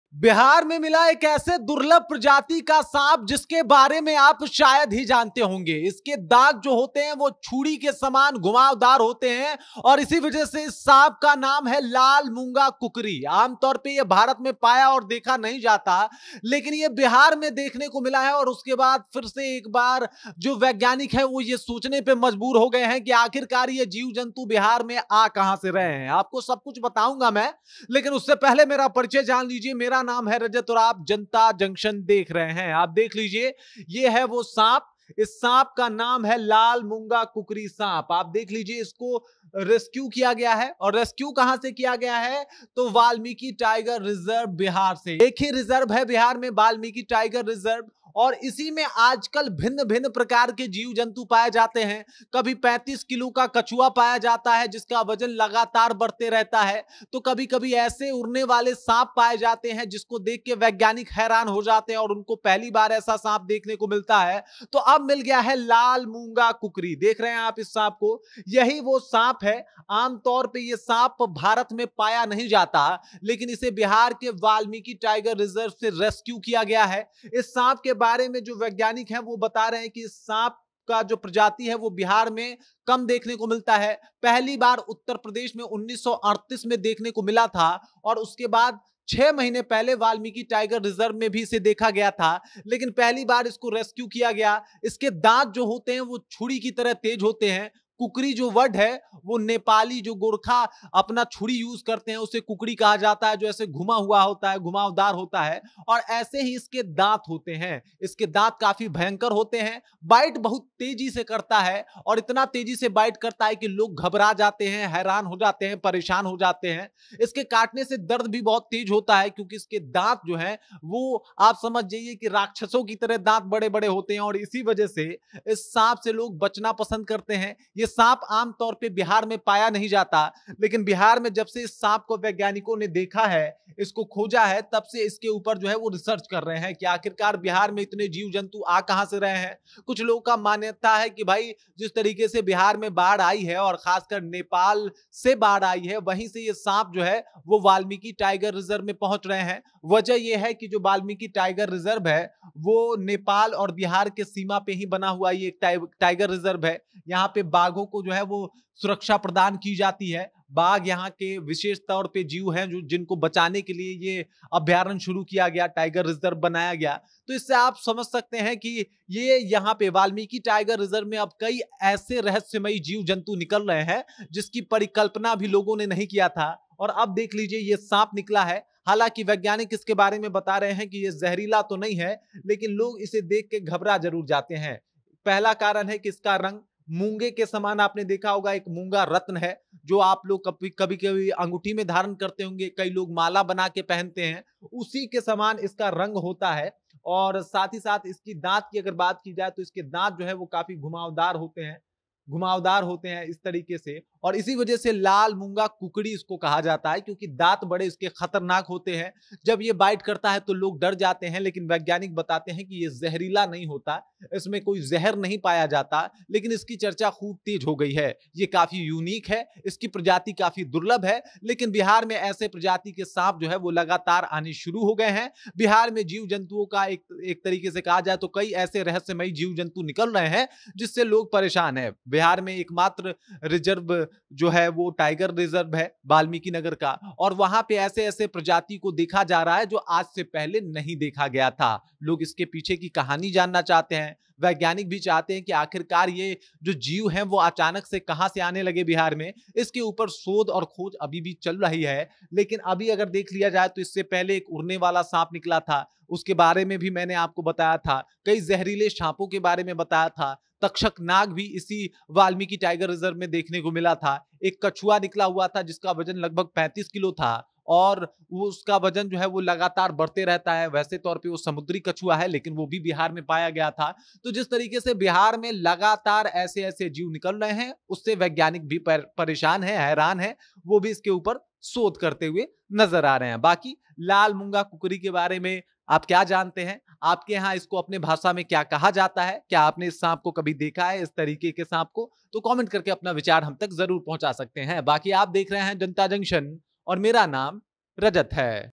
Bihar के Valmiki Tiger Reserve में मिला चा/कू की तरह तेज़ दांतो वाला snake, वैज्ञानिक दंग है,देखिए!
बिहार में मिला चाकू की तरह तेज दांतों वाला दुर्लभ प्रजाति का सांप लाल मूंगा कुकरी। चा/कू की तरह तेज दांत है इसके, भारत में नही देखने को मिलता ये सांप, वैज्ञानिक हैरान हैं,देखिए!